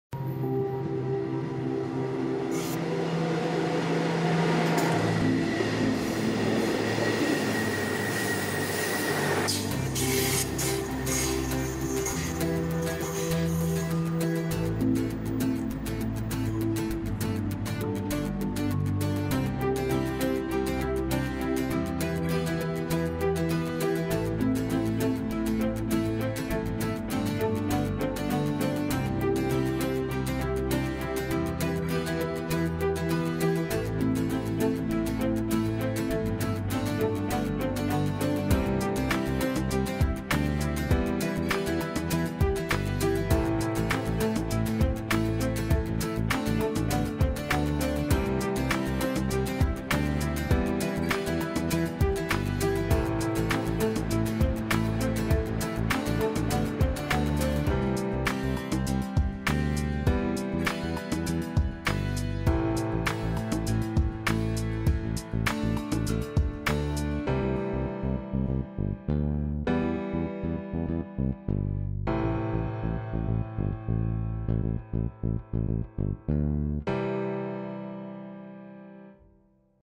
Welding work during major overhaul og train set - Projector
At Projector we are specialized in servicing train sets from end to end.
This video displays the welding work that takes place, when we are conducting a major overhaul on a train set.
We have an eye on the great whole and the smallest detail when you train is serviced. Everything is checked and optimized - thereby ensuring that your train and your business stays on track.